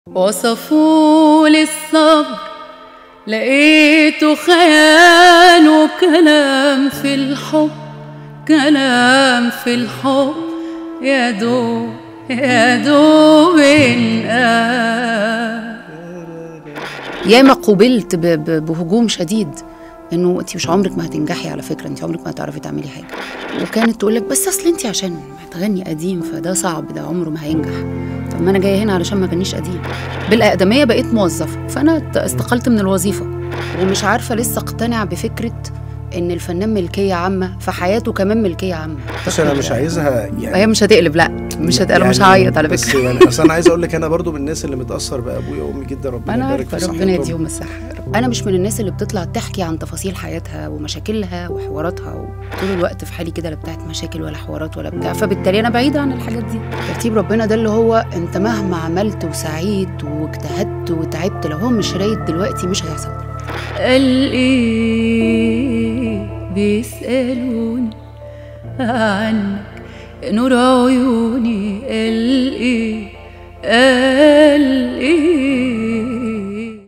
Ahmed aboulyazeed Bey2ool Promo l برومو حلقة المطربة مي فاروق .. ولحظة التحول في حياتها .. انتظرونا
انتظرونا غداً الثلاثاء الساعة 6.30 مساءً والحلقة الكاملة مع المطربة مي فاروق في بودكاست أبواليزيد بيقول ..

شاهدوا ايضاً حلقات سابقة من بودكاست ابواليزيد

Ahmed aboulyazeed |  جعل من عادل إمام بطلاً .. ونور الشريف عيشه في رعب .. المصور الصحفي عادل مبارز